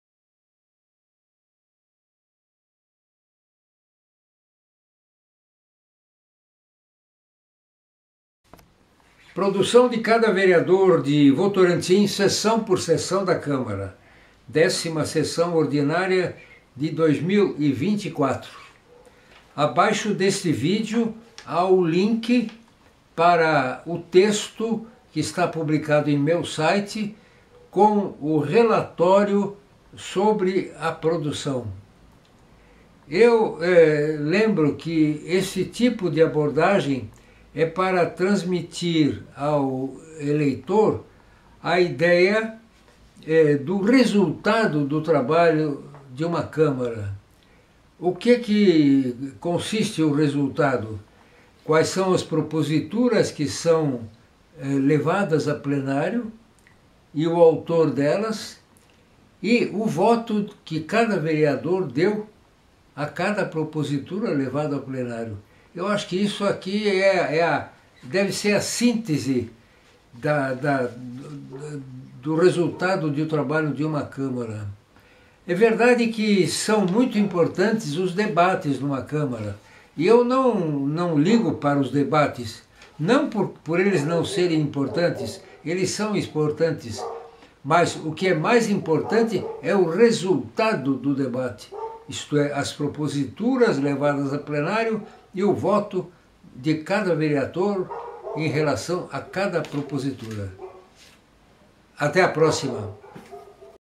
Produção de cada vereador de Votorantim, sessão por sessão da Câmara - 10ª Sessão Ordinária de 2024
PRODUÇÃO DE CADA VEREADOR DE VOTORANTIM, SESSÃO POR SESSÃO DA CÂMARA